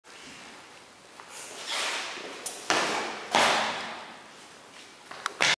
猛虎硬爬山
虎掌打⇒突き⇒肘打。合気道サンボの当身の威力を高めます。